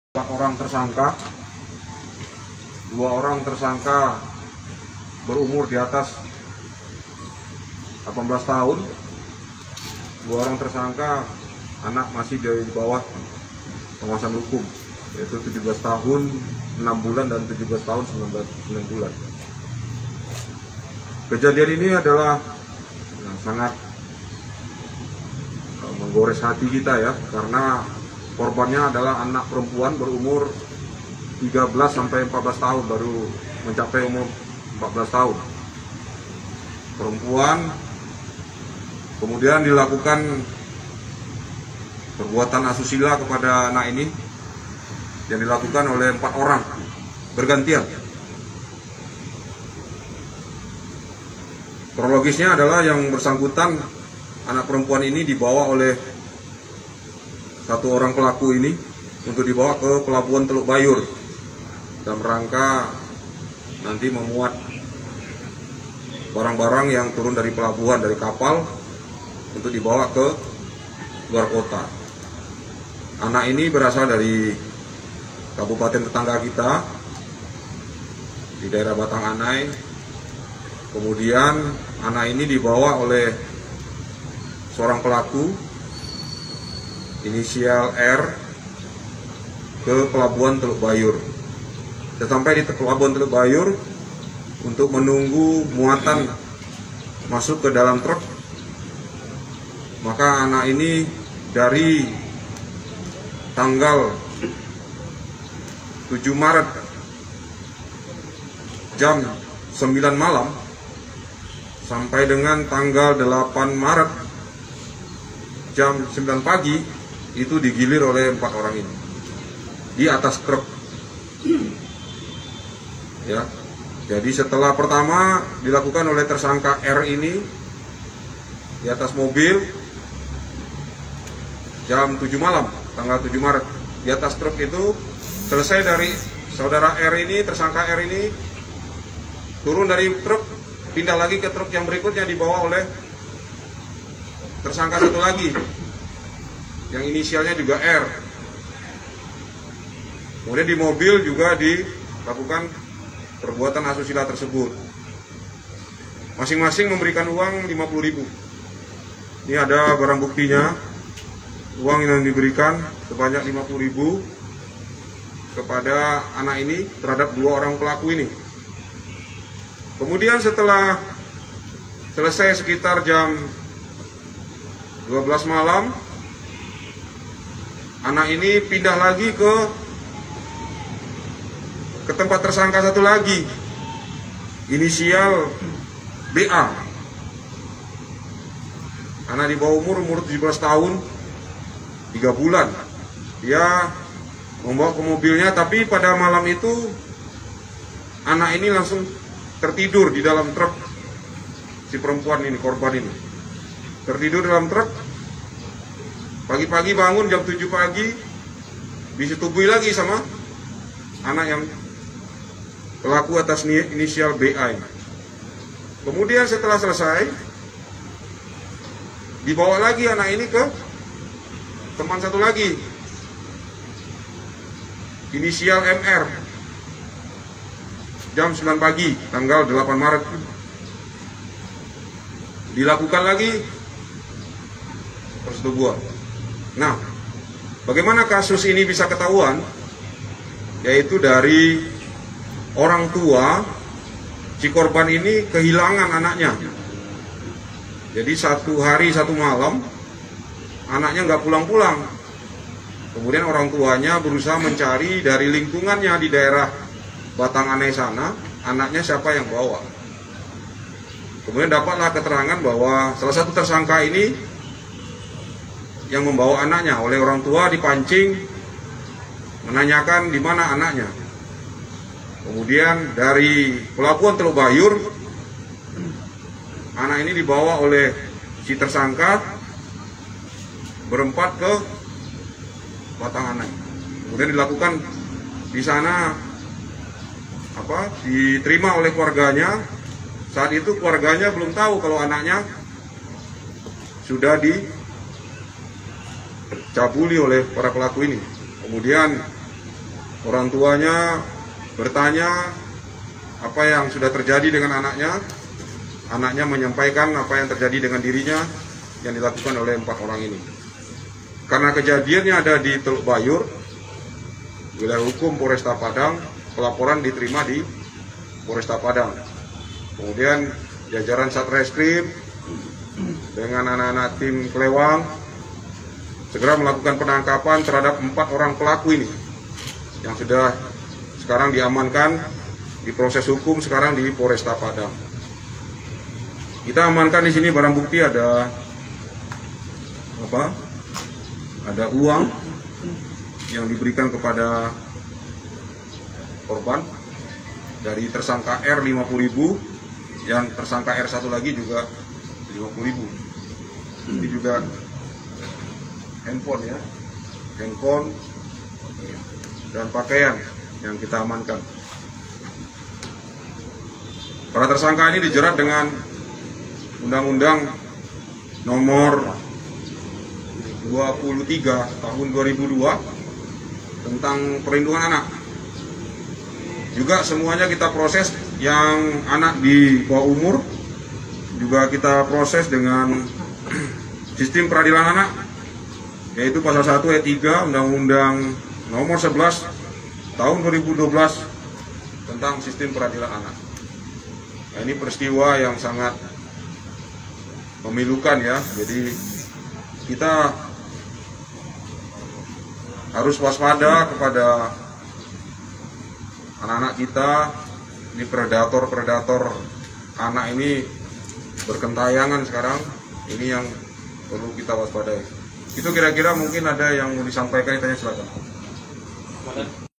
Miris, Anak di Bawah Umur Dicabuli Bergilir oleh Sopir Truk di Padang
Seorang Remaja Perempuan Dilecehkan Secara Bergilir oleh 4 Orang, Dua Pelaku Masih di Bawah Umur

Seorang remaja perempuan diduga mendapat perlakuan kejahatan seksual oleh sopir truk di Kota Padang, Provinsi Sumatera Barat (Sumbar), Rabu (9/3/2022).

Perkara kekerasan seksual ini ditangani oleh Unit PPA Satreskrim Polresta Padang.

Sedangkan pelaku sudah diamankan oleh jajaran tim Klewang Polresta Padang bersama dengan Unit PPA Polresta Padang.

Kapolresta Padang, Kombes Pol Imran Amir, mengatakan bahwa pelaku kejahatan seksual ini ada sebanyak empat orang.

"Pelakunya ada empat orang yang terdiri dari dua orang dewasa dan dua orang masih anak di bawah umur," kata Kombes Pol Imran Amir.

Kata dia, keempat pelaku melakukan persetubuhan terhadap anak di bawah umur.

"Pelaku dewasa berinisial RS panggilan R (29) dan RFN panggilan R (19). Kedua pelaku dewasa ini merupakan sppir truk," katanya.

Sedangkan pelaku yang masih di bawah umur atau anak berkonflik dengan hukum berinisial BA (17) dan MR (17)."Korbannya masih duduk di bangku Sekolah Dasar (SD) kelas lima," katanya.

Ia menjelaskan, untuk umur korban sudah besar, dan diduga telat masuk sekolah sehingga masih duduk di bangku SD."Untuk korban ini masih berumur 13 tahun berinisial AC," katanya.

Ia mengatakan, bahwa korban diduga dilecehkan secara bergilir oleh keempat terduga pelaku kejahatan.